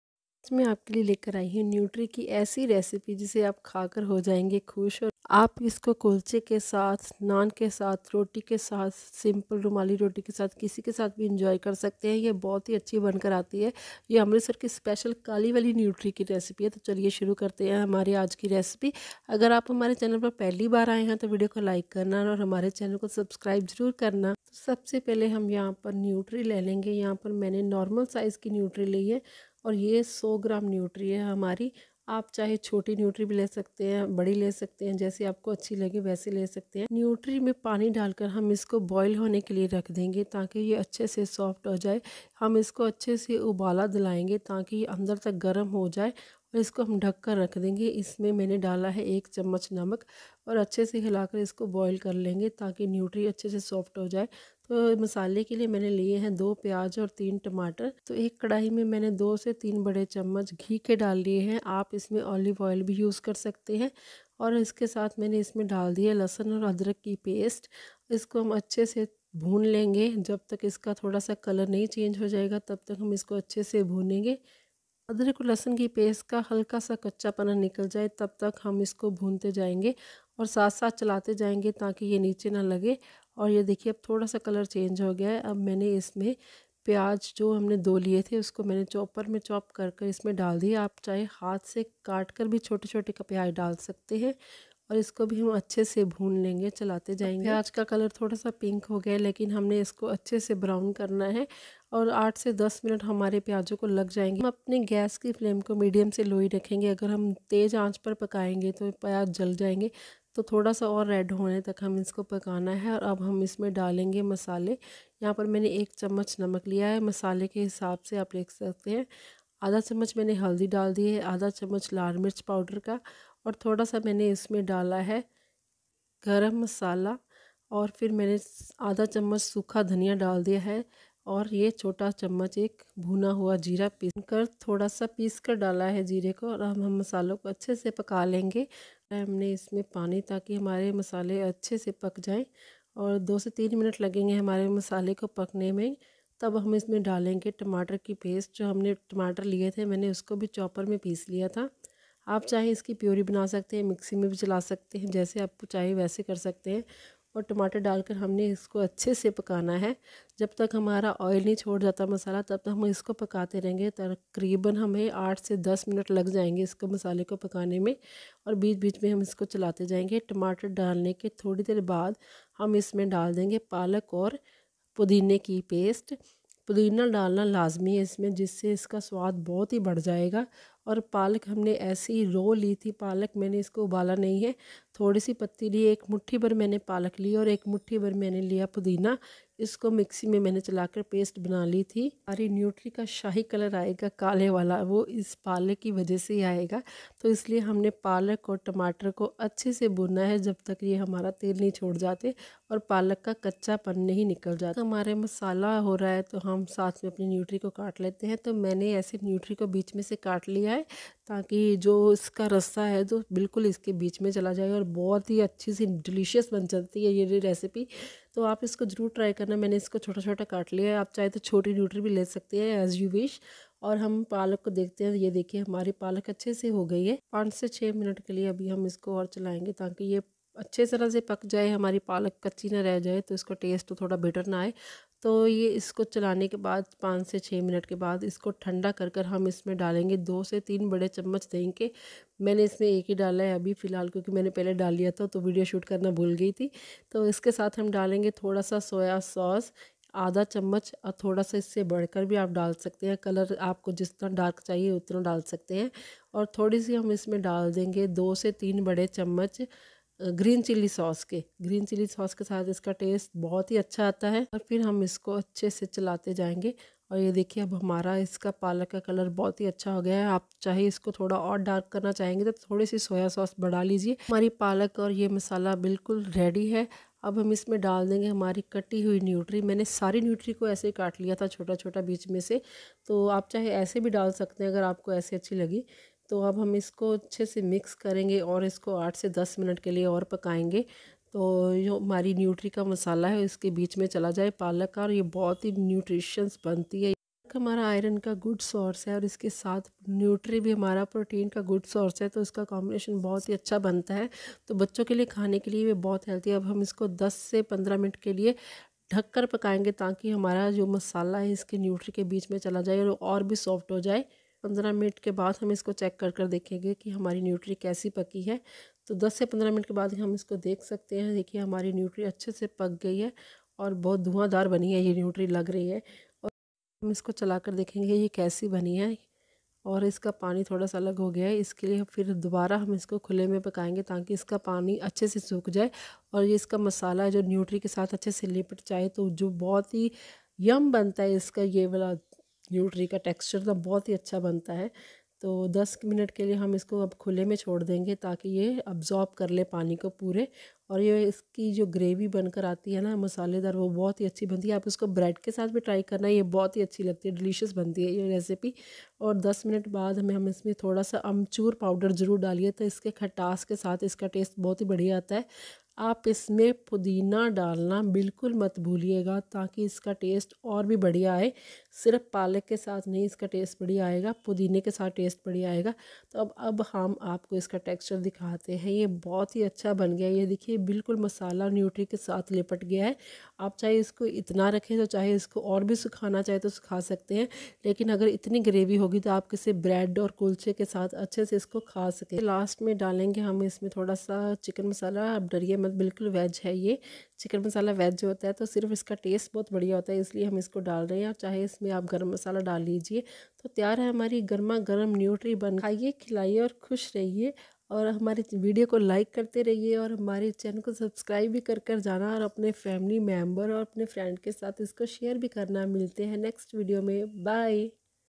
Nutri Recipe | Palak Nutri Recipe | Shahi Nutri
Hi friends ajj mein app k leye le kar aye hu amritsar ki mashoor palak aur nutri ki recipe.

Your Queries.